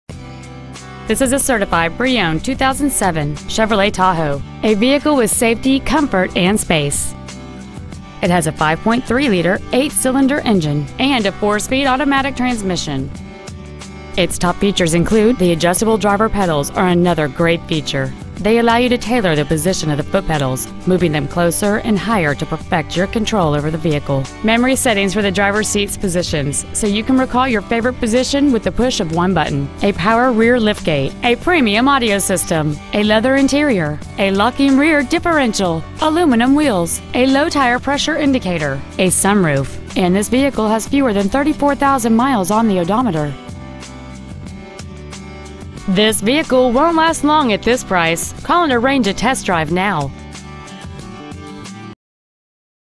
Used 2007 Chevrolet Tahoe Acworth GA
Year: 2007
 Make: Chevrolet
 Model: Tahoe
 Engine: Gas V8 5.3L/323
 Trans.: Automatic
 Exterior: Blue
 Miles: 33,426
 Interior: Light Gray

 http://

 Pre-Owned 2007 Chevrolet Tahoe Acworth GA